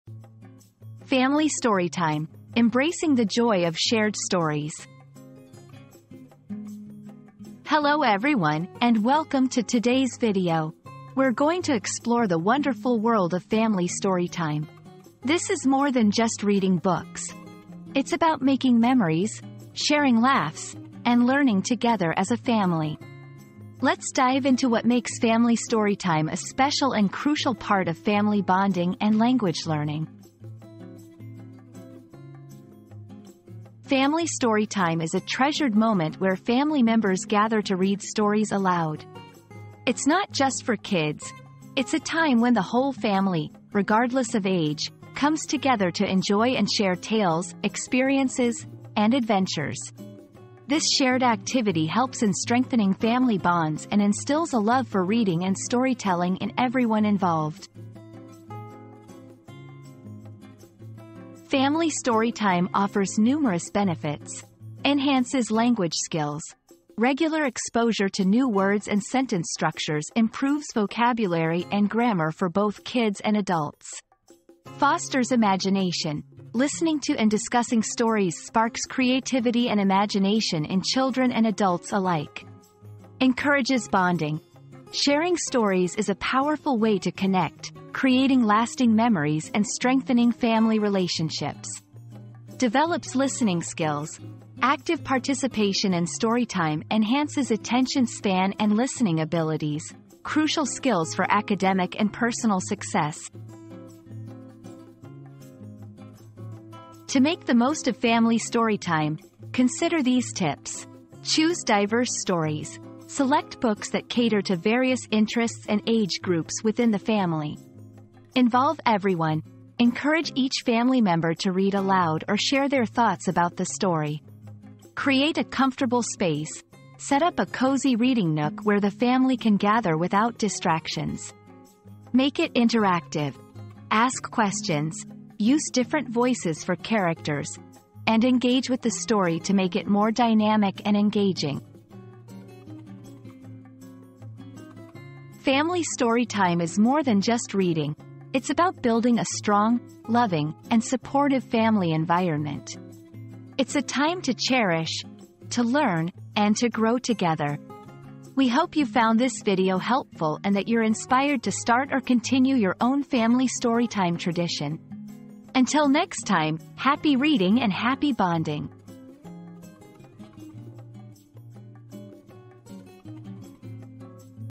Family Storytime: Embracing the Joy of Shared Stories
Embrace the Joy of Shared Stories with Family Storytime • Join us for Family Storytime and experience the pure delight of sharing stories together. Discover the magic that unfolds as we come together as a family to explore the world of imagination, laughter, and love. Whether it's a classic tale or a new adventure, let's create unforgettable memories and foster a lifelong love for storytelling. Don't miss out on this heartwarming experience!

00:00 • Introduction - Family Storytime: Embracing the Joy of Shared Stories
00:33 • Understanding Family Storytime
01:00 • Benefits of Family Storytime
01:46 • Tips for a Successful Family Storytime